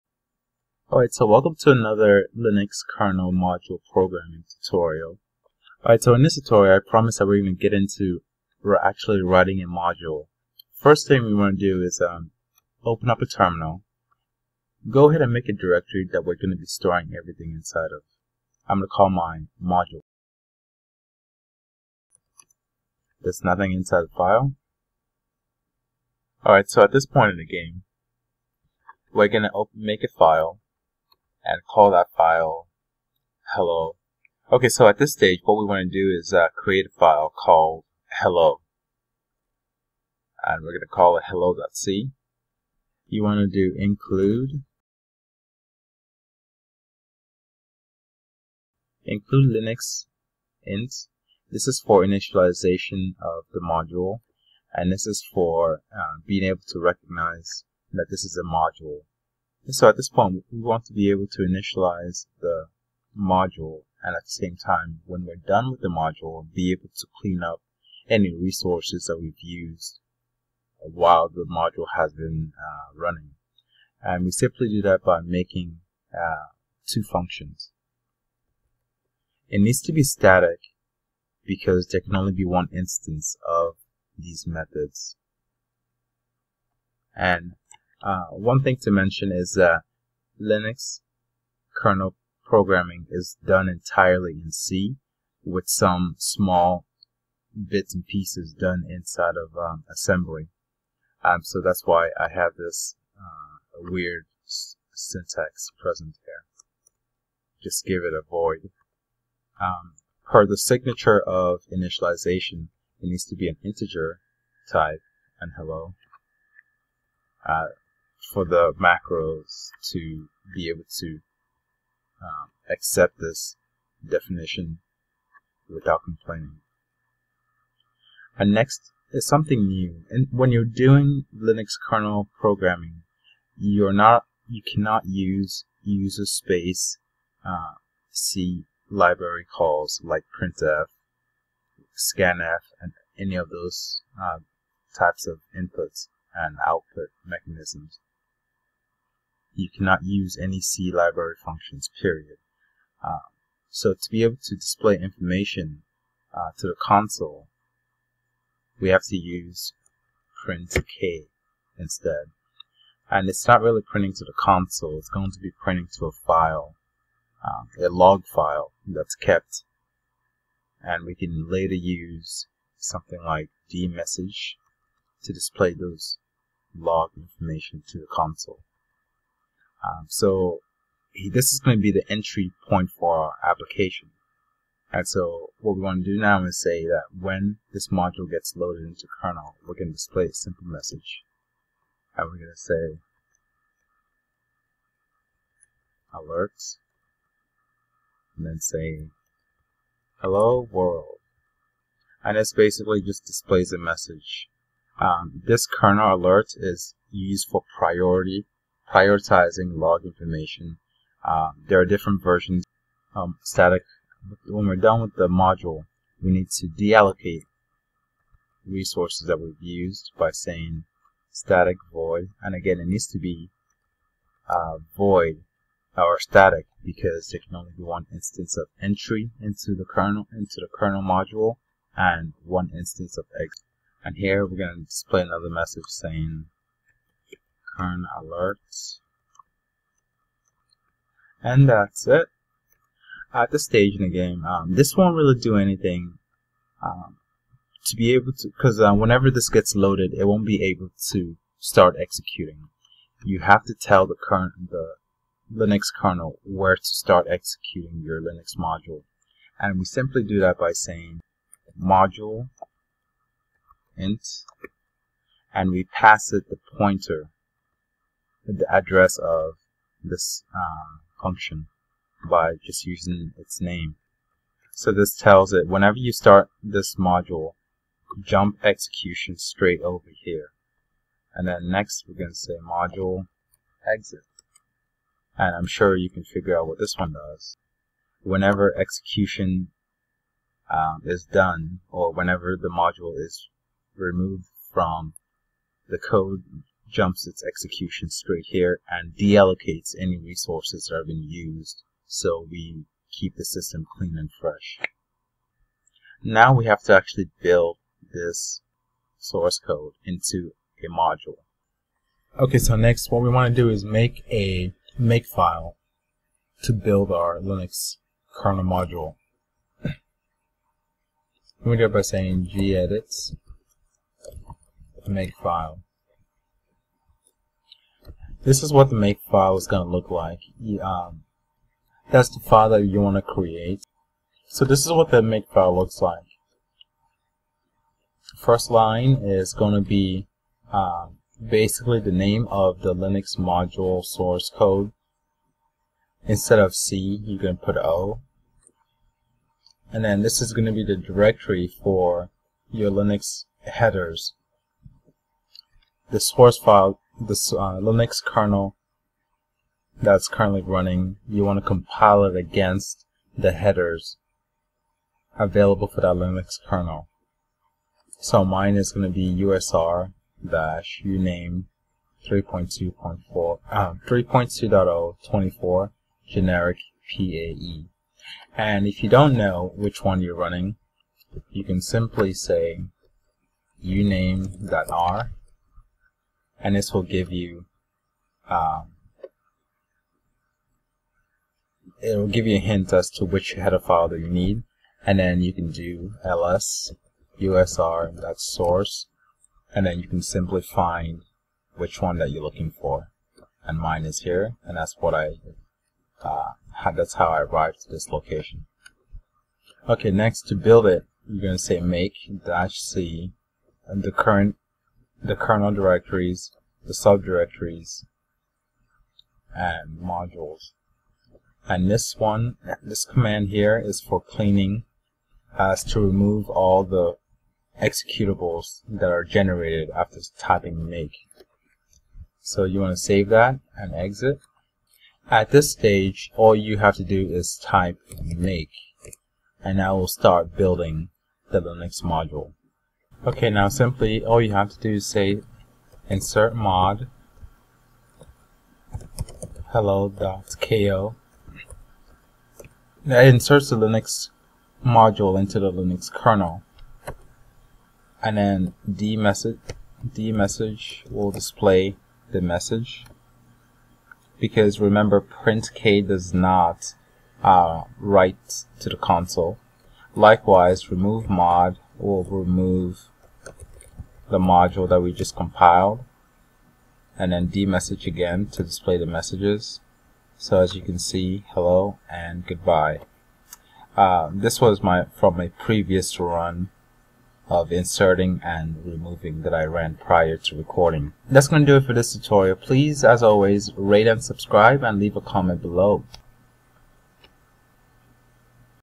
Linux Kernel Module Programming - 03 Coding, Compiling the Module
In this video we get into actually writing a simple hello world version of a linux module.

Recall a kernel module is used to extend the functionally of an exisiting system.  Our, though not very complex and useful, introduces of elementary comcepts.